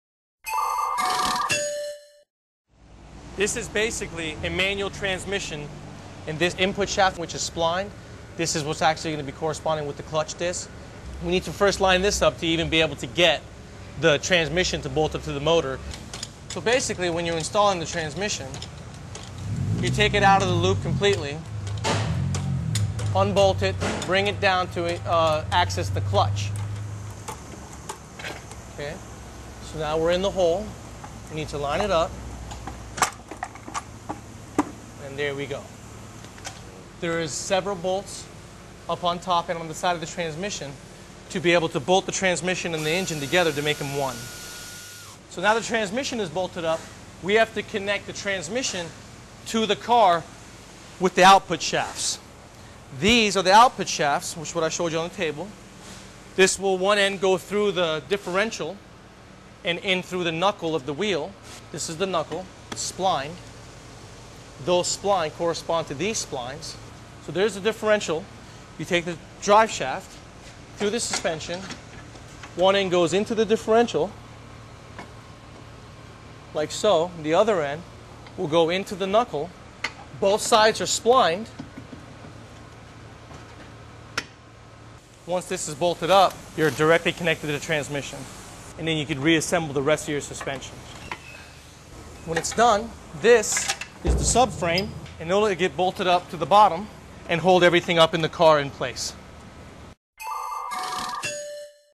How Manual Transmissions Work: Installation
There's something about driving a stick-shift car that's both empowering and invigorating. Find out all about manual transmissions.